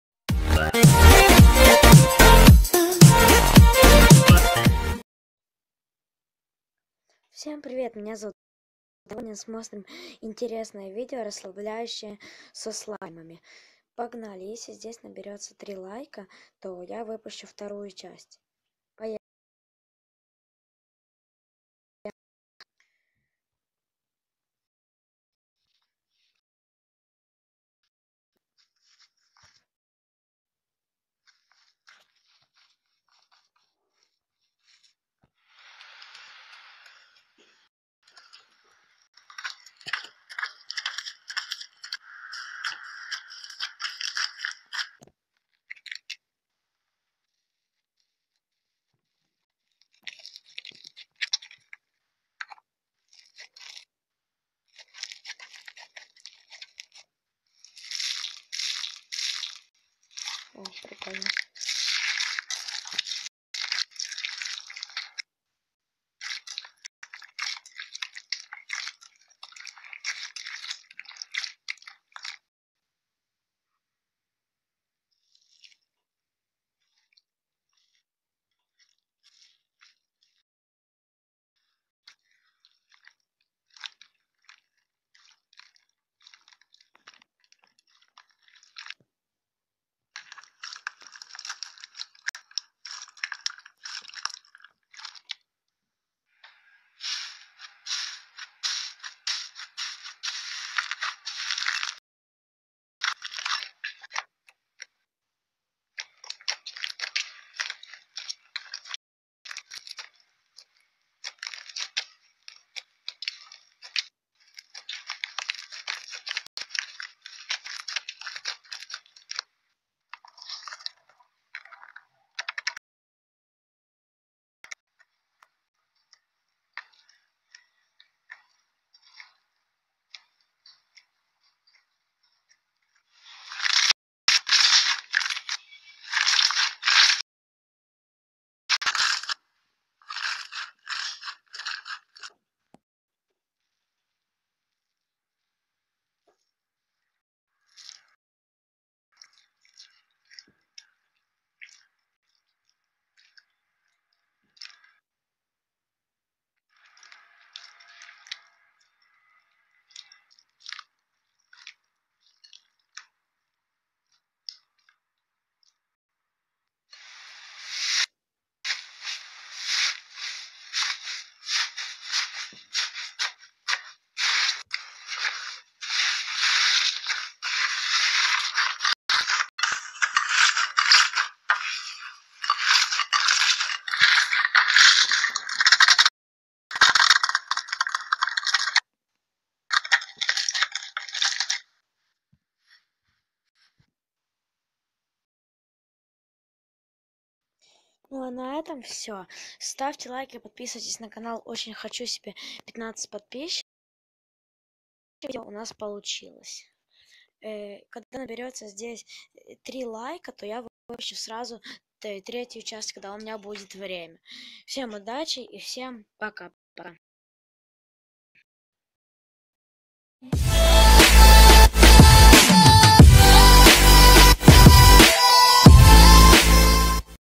СЛАЙМЫ.ПРИЯТНЫЕ ЗВУКИ 1 ЧАСТЬ
СЛУШАЕМ ПРИЯТНИЕ ЗВУКИ КОГДА ЖМЯКАЮТ СЛАЙМ